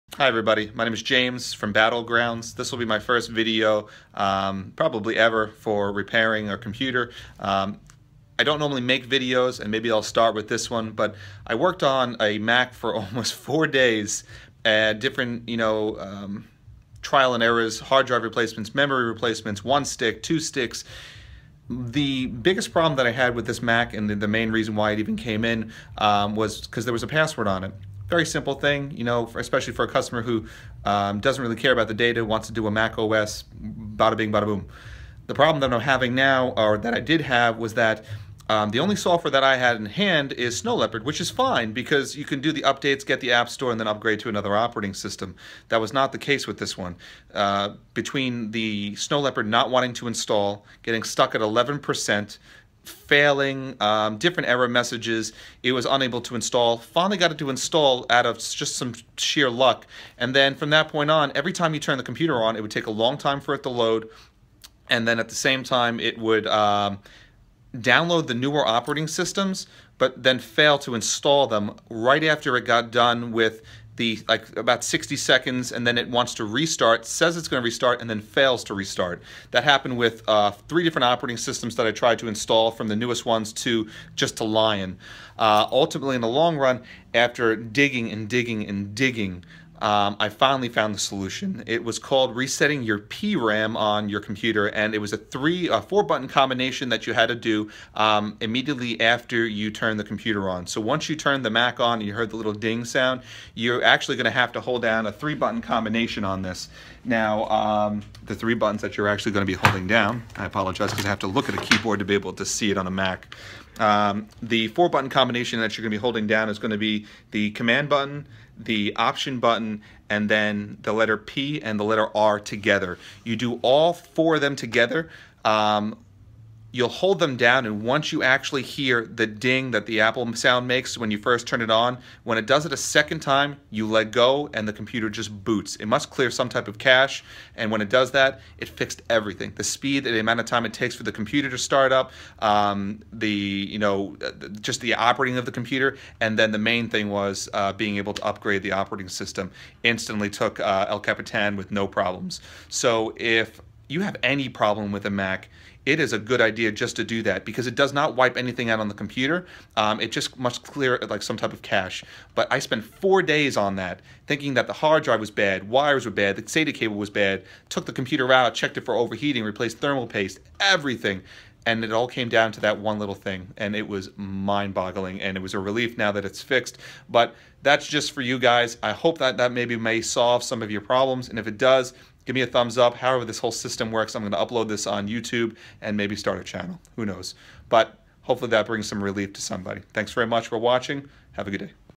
BG fixed it: Mac OS snow leopard install error. Mac OS upgrade failure.
When you have a Mac that fails to install the OS with the CD.
When you have a mac that gets stuck or crashes at 11 minutes remaining.
When you try and download and install a new mac OS from the app store and it has an error when trying to restart after extracting